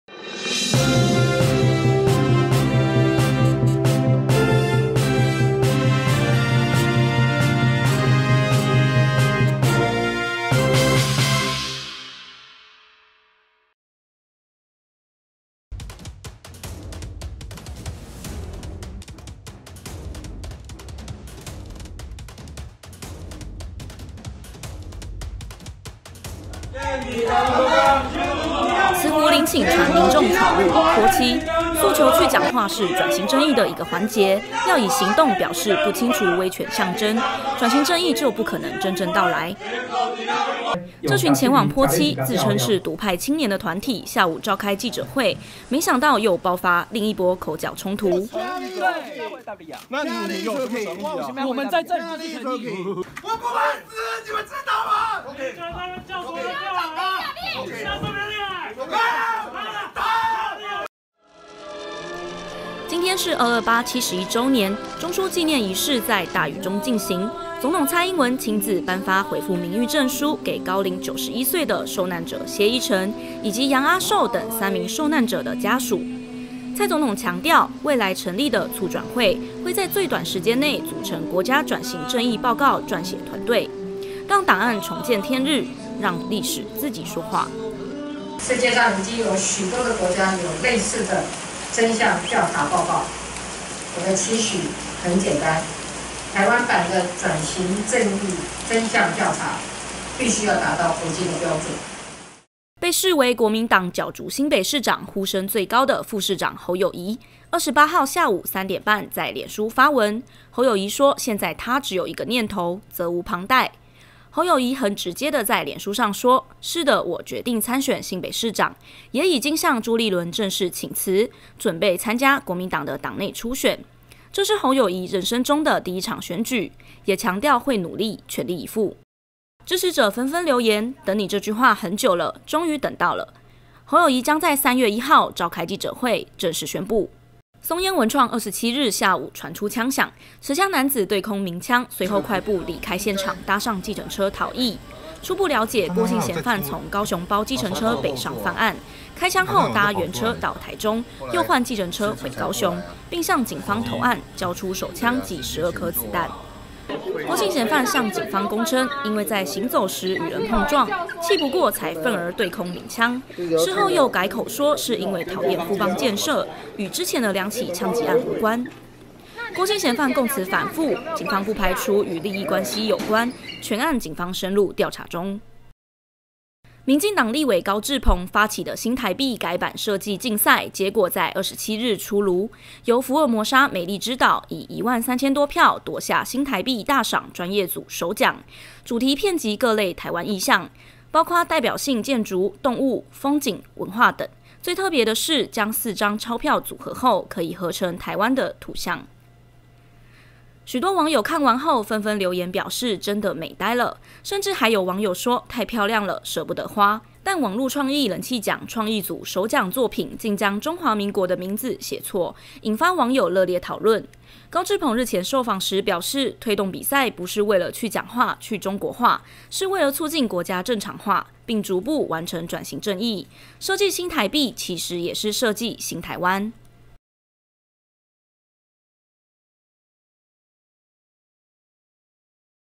【02/28】每日焦點新聞
慈湖陵寢被侵入 驚傳蔣中正靈櫬被潑紅漆

總統宣示 台灣版轉型正義調查要達國際標準

北市警漏夜訊松菸槍手 懷疑與前2案有關

「新新台幣」設計出爐 網路人氣組國號錯字